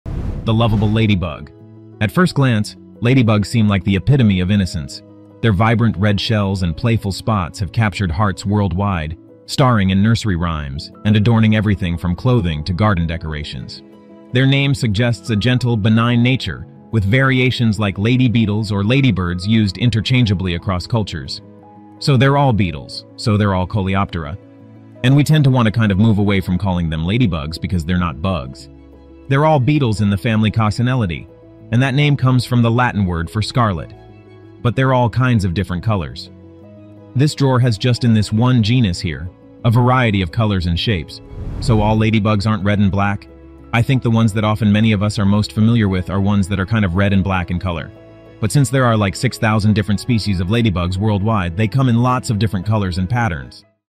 Discover the Hidden Killer Secret of Ladybugs
In this captivating short, we'll explore the lesser-known aspects of ladybugs, shedding light on their remarkable survival strategies and the role they play in their ecosystems. 🌍 Prepare to be amazed as we reveal the hidden killer secret that lurks beneath their adorable exterior.

From their intricate life cycles to their surprising predatory behaviors, this video is packed with eye-opening insights that will leave you viewing ladybugs in a whole new light. 🌟

Join us on this journey of discovery as we unravel the mysteries of one of nature's most beloved creatures. Don't miss out—hit play now and let the adventure begin! 🎥✨